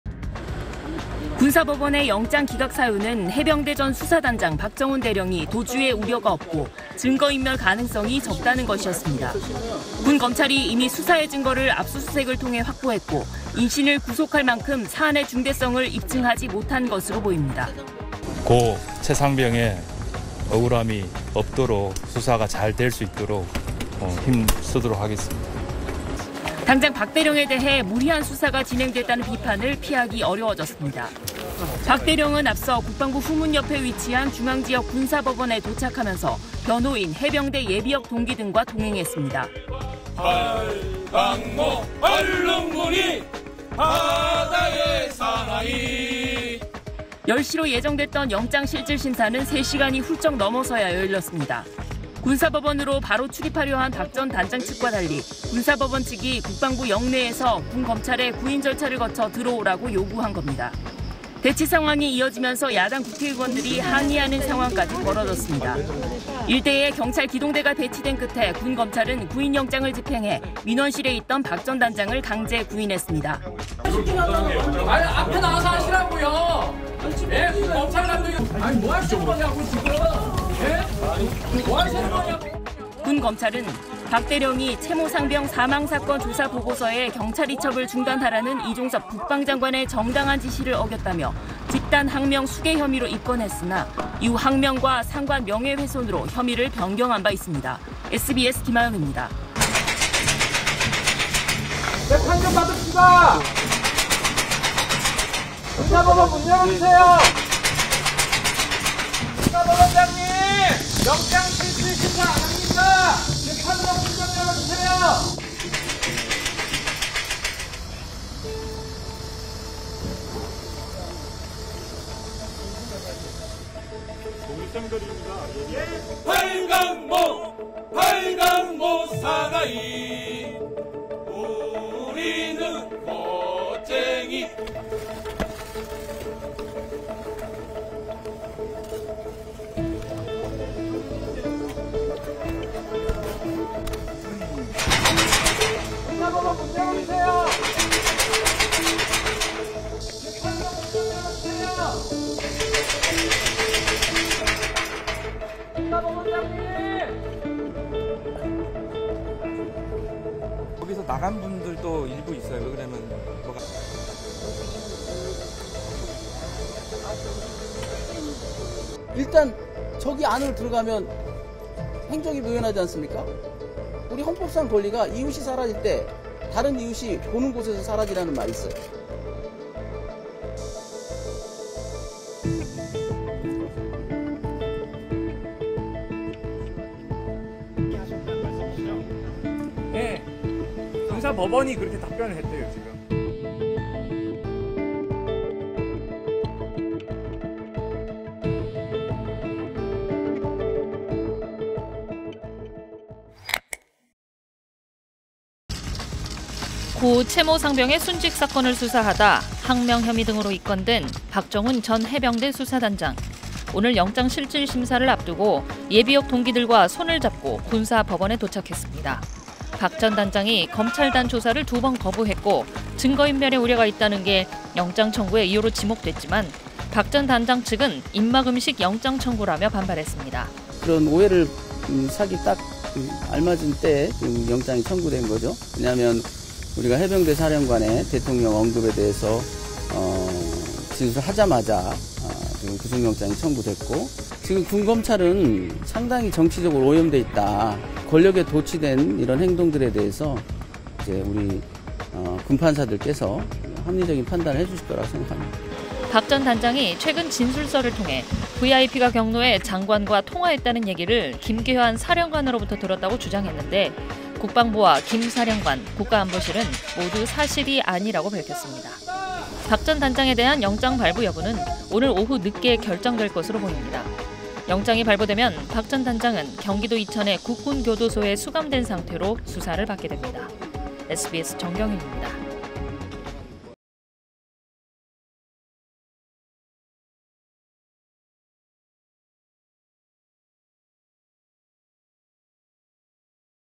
"하루 빨리 외압 실체 규명되길"..박정훈 전 수사단장 영장 기각 "무리한 수사" 역풍 맞나? / SBS / 모아보는 뉴스
00:00 박정훈 전 수사단장 영장 기각…출입 방식 놓고 '실랑이'
01:40[영상] "재판 좀 받읍시다! 문 열어주세요!" 굳게 닫힌 군사법원 정문…강제 구인된 박정훈 전 해병대 수사단장
03:26 "군 검찰은 정치적으로 오염됐다"…동기들 끌어안고 영장심사 출석